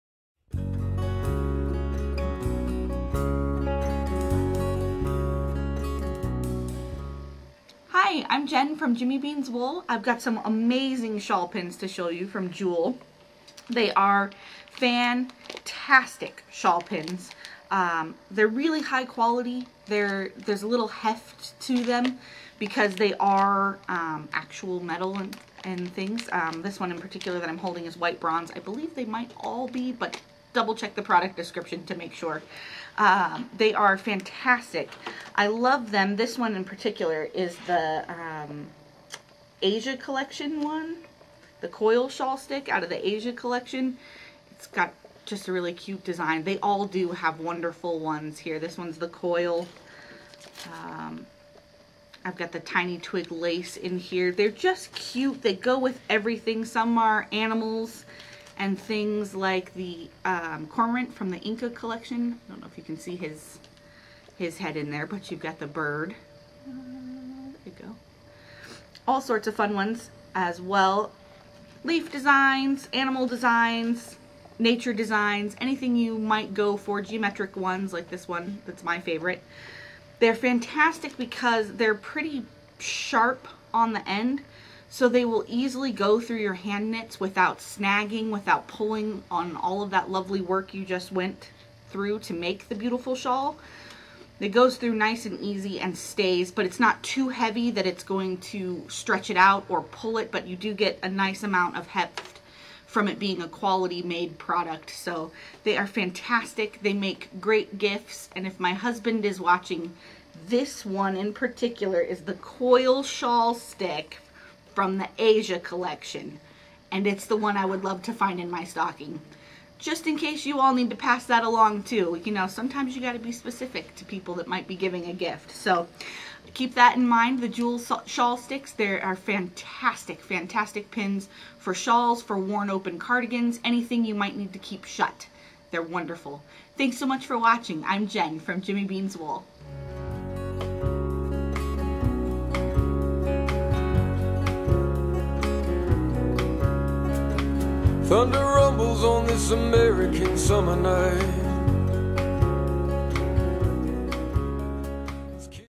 Jul Shawl Sticks Review
Jenn from Jimmy Beans Wool reviews Jul Shawl Sticks.  Jimmy Beans Wool is your LYS... online!